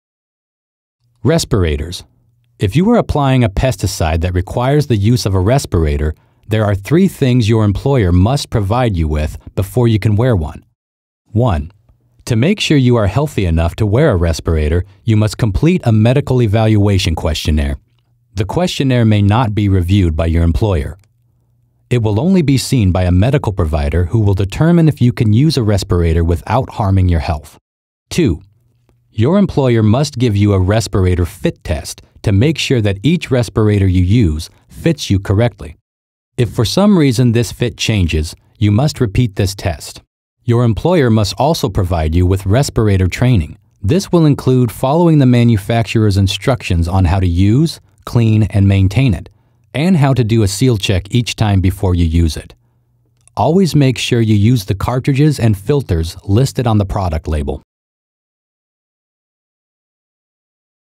Reviews the requirements for respirator use under the worker protection standards
For Pesticide Handlers

This video alone does not meet the annual training requirements under the Worker Protection Standard (WPS). It can be used as part of a broader training program, which must be EPA-approved in its entirety. Handlers